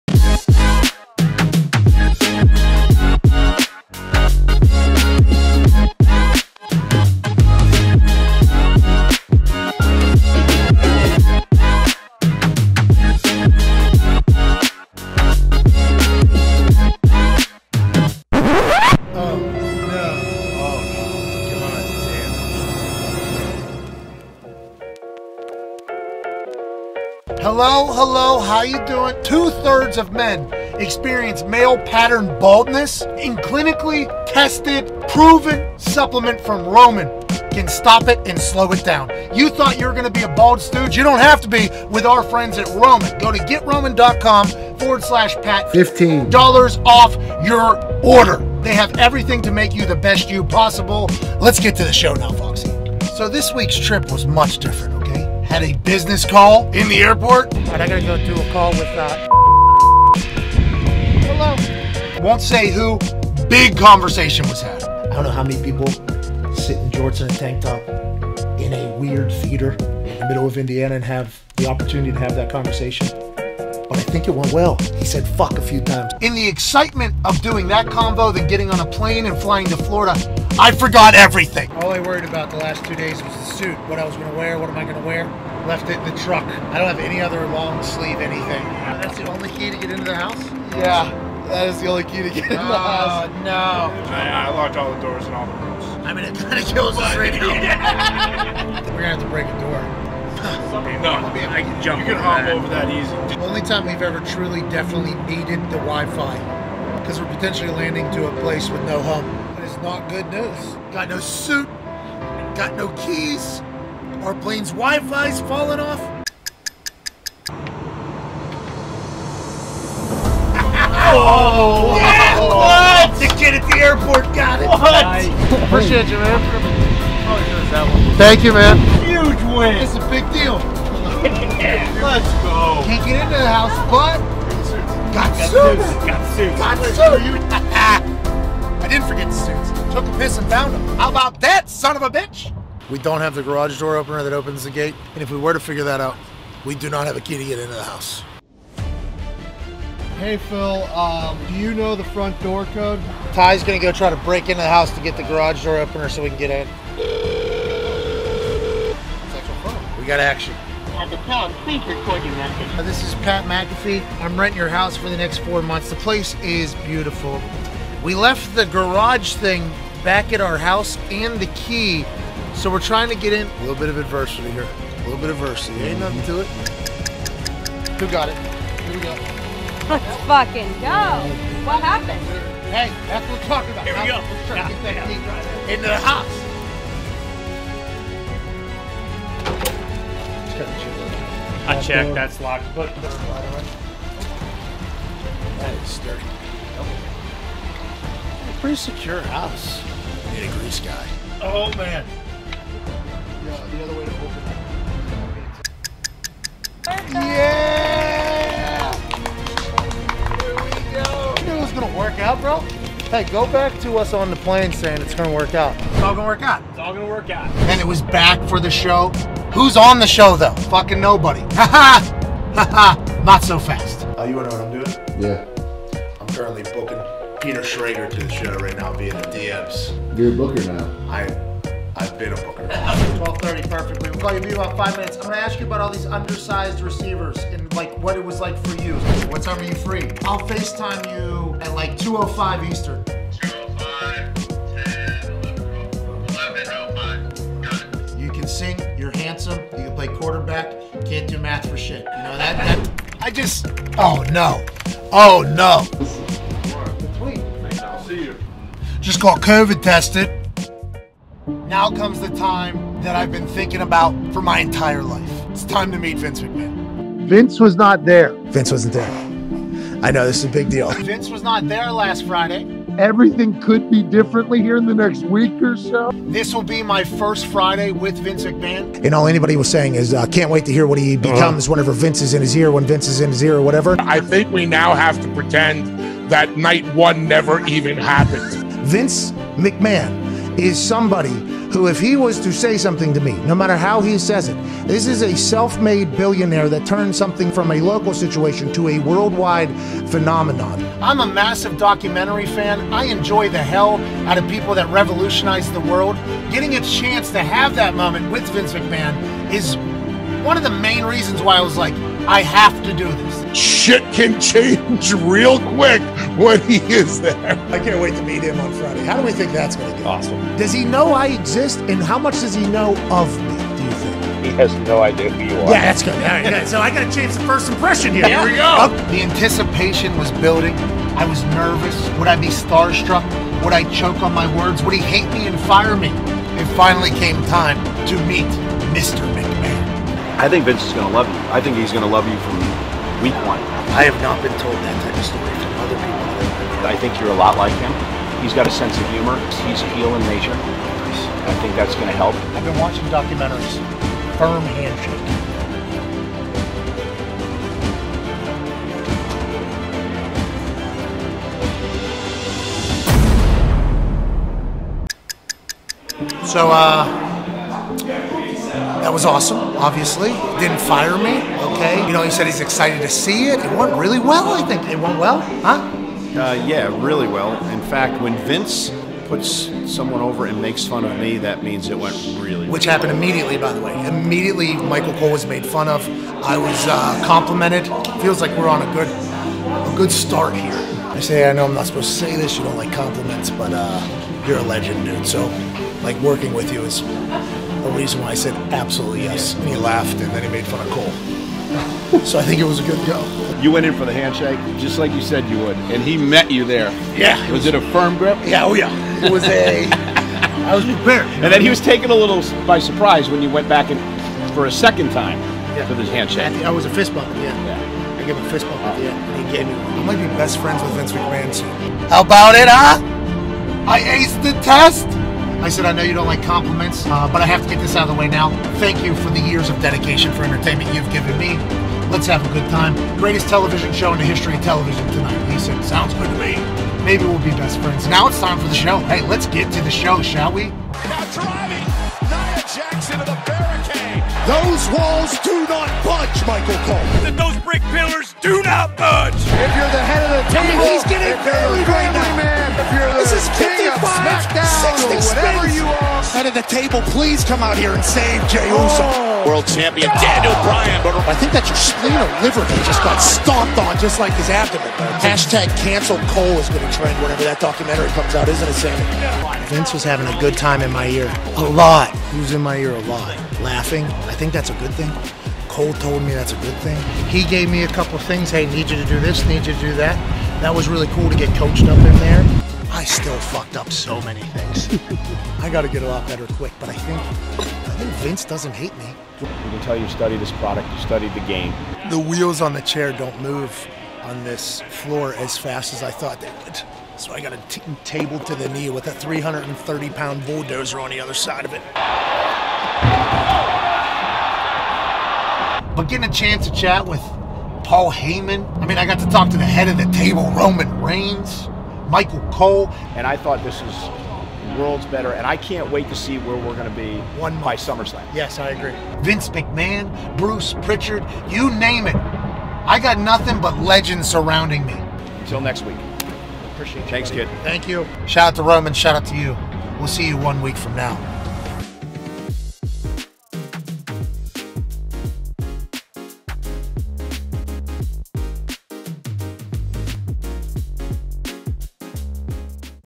Pat McAfee Meets Vince McMahon, Gets Locked Out Of New Studio! | Mr. Friday Night Vlog #2
Some lifelong dream stuff going on in this vlog. Be sure to tweet @EvanFoxy and thank him for the hard work that went into this.

for a free online visit and free 2-day shipping.